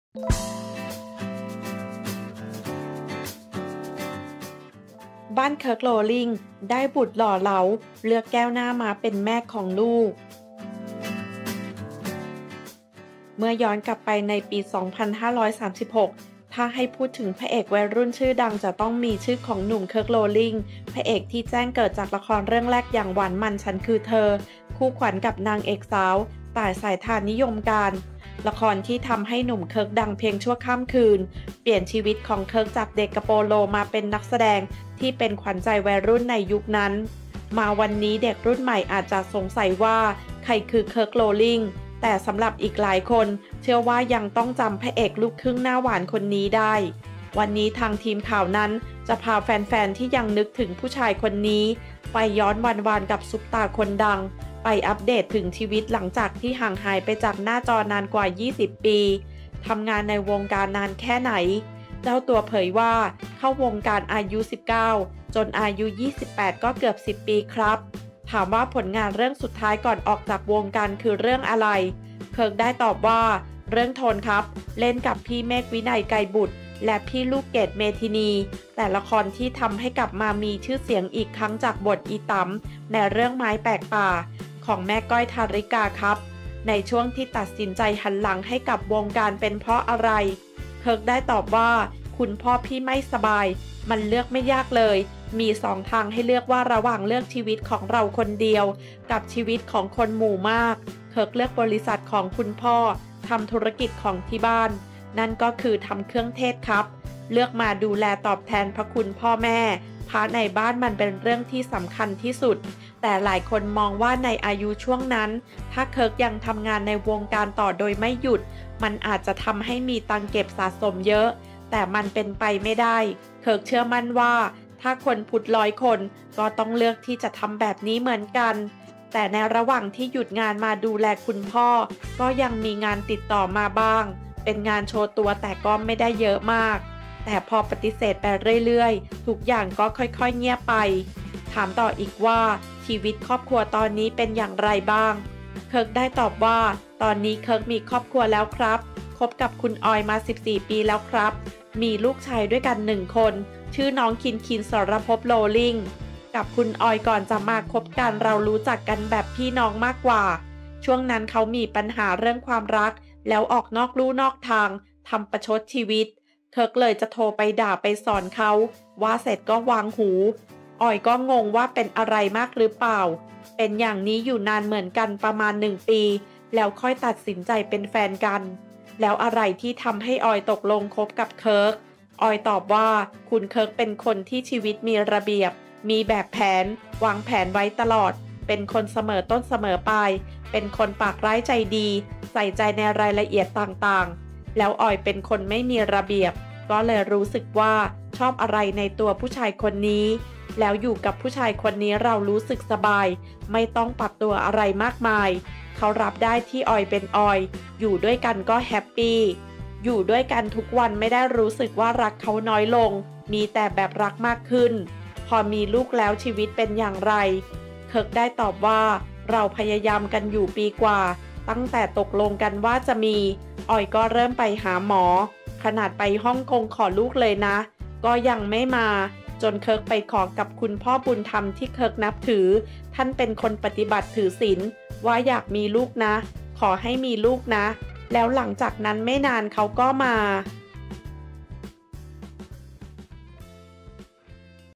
มีลูกกับ แก้วหน้าม้า! เคิร์ก โรลลิ่ง ได้บุตรหล่อเหลา เลือก ออย สิริมา เป็นแม่ของลูก
เมื่อย้อนกลับไปในปี 2536 ถ้าให้พูดถึงพระเอกวัยรุ่นชื่อดังจะต้องมีชื่อของหนุ่ม เคิร์ก โรลลิ่ง พระเอกที่แจ้งเกิดจากละครเรื่องแรกอย่าง หวานมันส์ ฉันคือเธอ คู่ขวัญ

กับนางเอกสาว ต่าย สายธาร นิยมการณ์ ละครที่ทำให้หนุ่มเคิร์กดังเพียงชั่วข้ามคืน เปลี่ยนชีวิตของเคิร์กจากเด็กกะโปโลมาเป็นนักแสดงที่เป็นขวัญใจวันรุ่นในยุคนั้น